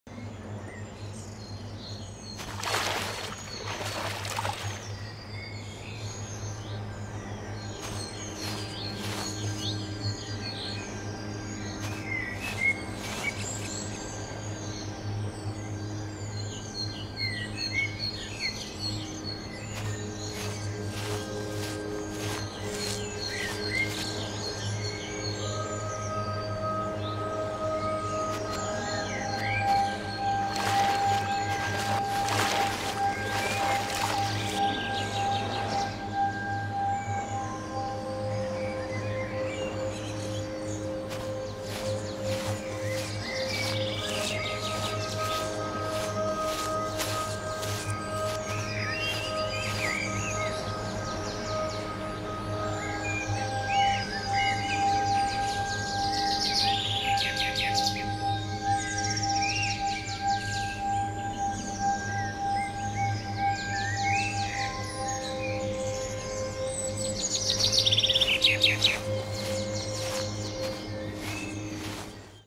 BGE Realistic Forest Environment
I added wind to the trees and the grass in my Landscape demo.
The wind makes it more lify and interesting.
I also baked the shadows to improve the performance.
Further uptdates may include:
-animals (Birds, Fish, ...)